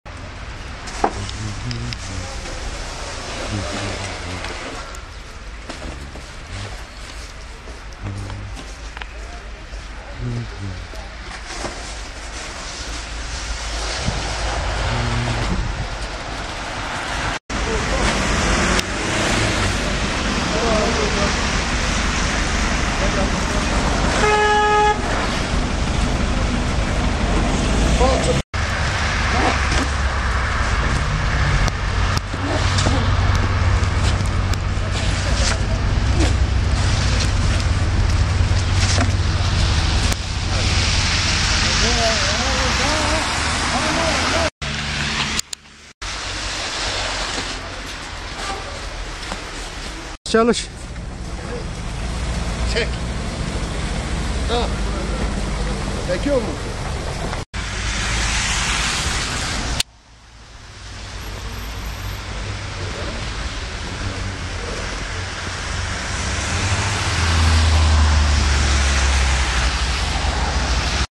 Ağrı'da kar yağışı || Ağrı'da karla mücadele - 2 Kasım 2016
Ağrı kar yağışı devam ediyor.  Ağrı'da etkili olan Kar yağışı sebebiyle kapanan 255 köy ve Mahalle yolu yeniden ulaşıma açıldı.